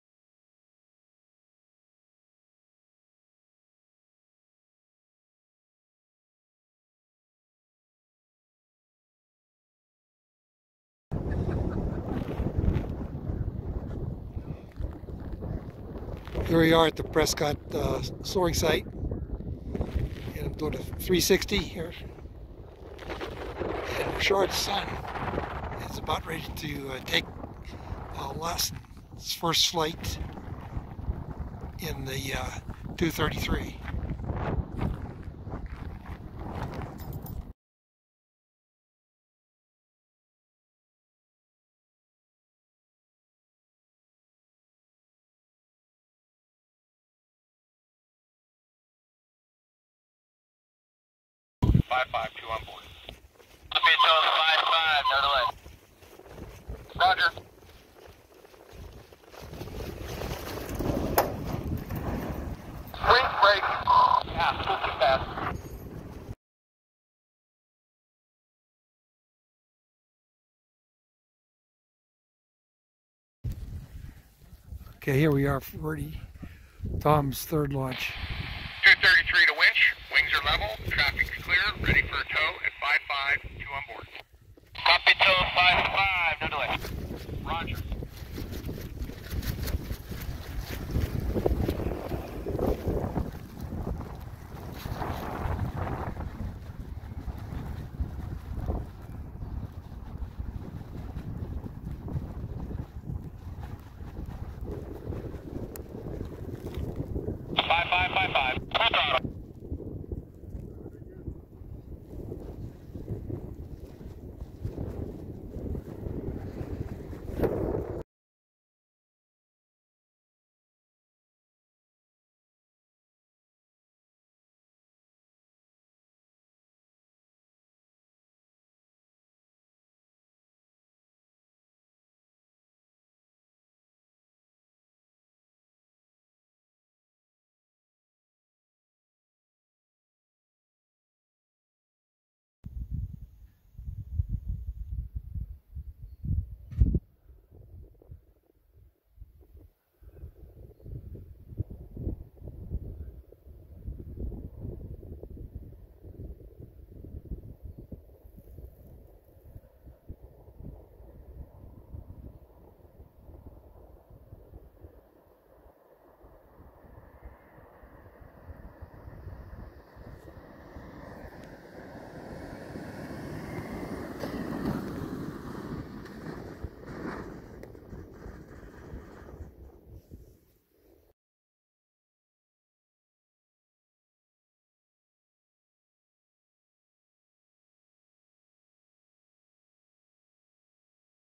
Ground Launch GliderTraining of February 12, 2021
While training for using a winch to launch gliders we had a winch line break that ended fine also at the end of the day the instructor landed the glider on a short runway bounded by three fences.  You can see the winch, the weak link and a repaired winch line made of steel.